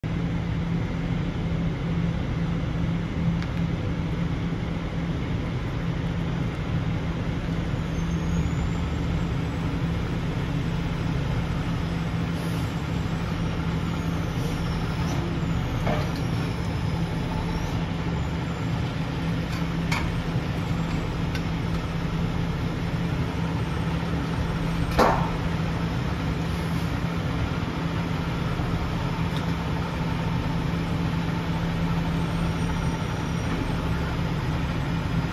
Landoll 850XT Loading the Shawshank Redemption bus
We took this bus from Montgomery, AL to the Shawshank Redemption museum in Ohio.